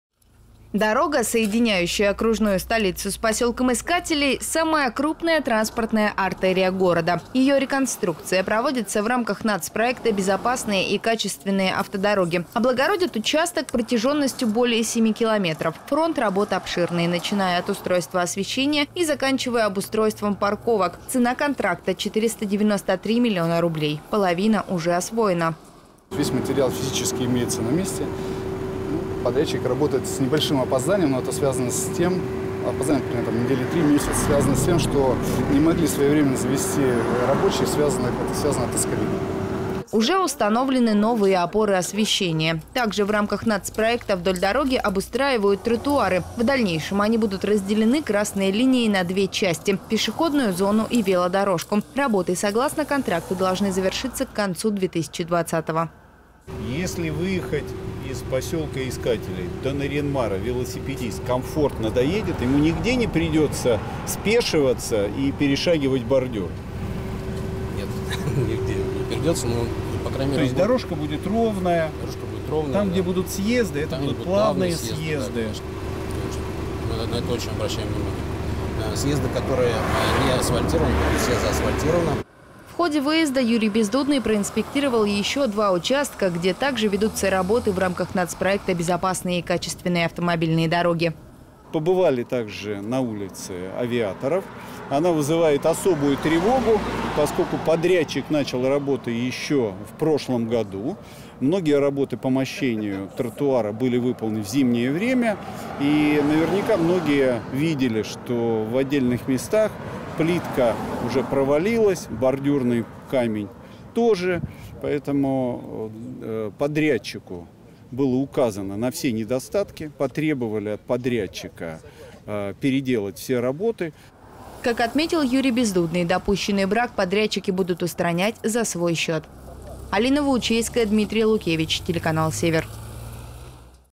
РЕЙД ПО ДОРОГАМ
Из Искателей в Нарьян-Мар - пешком или на велосипеде. На главной дороге окружной столицы продолжаются работы по реконструкции в рамках национального проекта "Безопасные и качественные автодороги". Объекты проинпектировал врио губернатора НАО Юрий Бездудный.